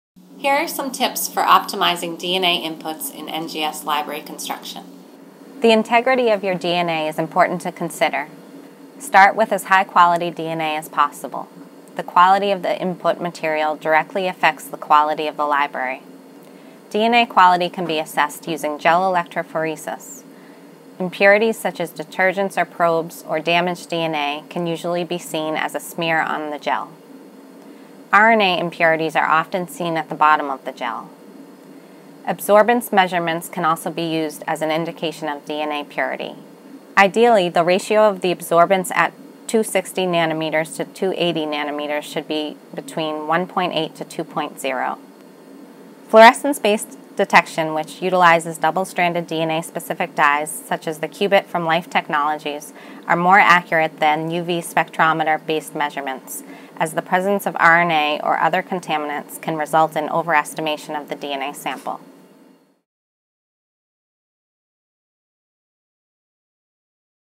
Tips for Optimizing DNA Inputs in NGS Library Construction
Optimizing DNA inputs for NGS library prep is an important step to ensure a high-quality library. Start by following these tips.